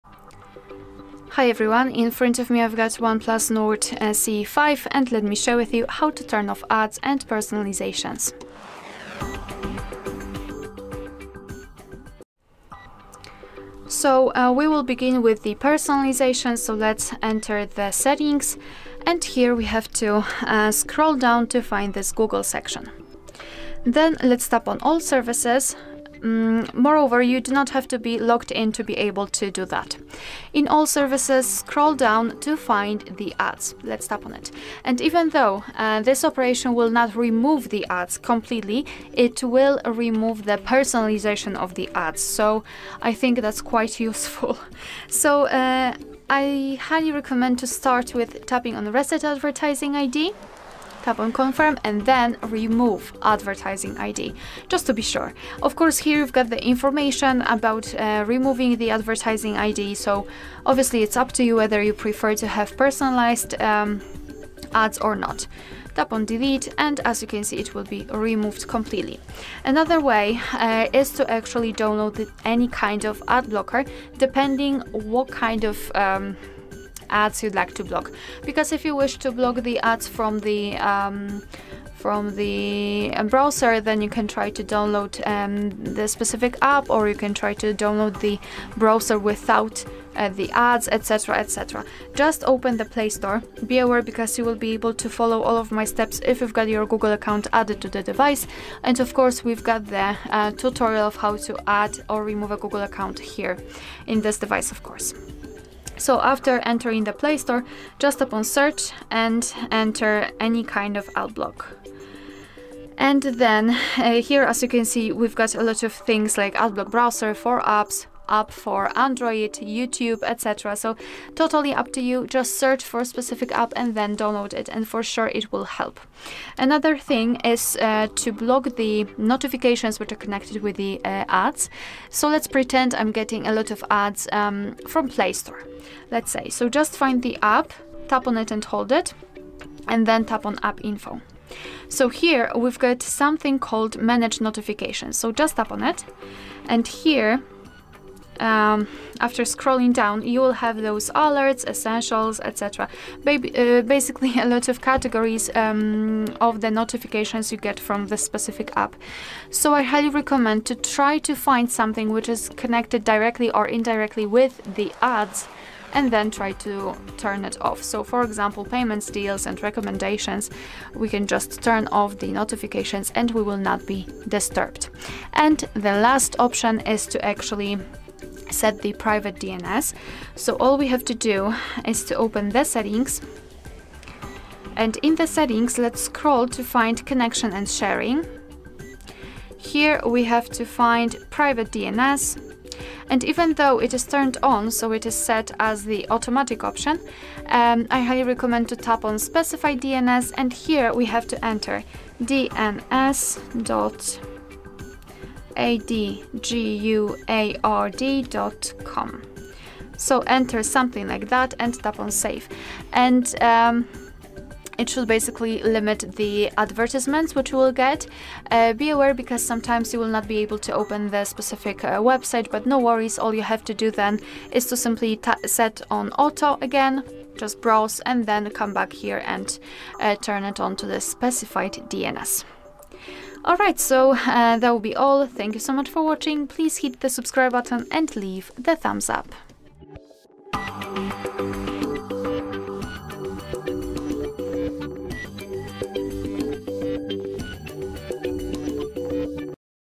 OnePlus Nord CE 5 – How to Turn Off Ads and Personalization
If you want to reduce or turn off ads and ad personalization on your OnePlus Nord CE 5, this video will show you all the steps you need. Learn how to limit personalized ads, reset your advertising ID, block ad notifications, use ad blocker apps, and set up Private DNS to minimize unwanted ads on your device. We’ll walk you through the settings menu, Google ad preferences, Play Store options, and more, so you can enjoy a cleaner and less distracting experience on your OnePlus Nord CE 5. Whether you’re tired of pop-ups, want to stop ad tracking, or just want more privacy, these tips will help you control ads on your phone.

How to turn off ads on OnePlus Nord CE 5?
How to disable ad personalization on OnePlus Nord CE 5?
How to block notifications from ad apps on OnePlus Nord CE 5?

0:00 Introduction
0:08 Open Settings and Find Google Section
0:23 Access Ad Personalization Settings
0:45 Reset and Remove Advertising ID
1:09 Using Ad Blocker Apps from Play Store
2:12 Block Ad Notifications from Apps
3:00 Set Up Private DNS for Ad Blocking
3:43 Tips and Troubleshooting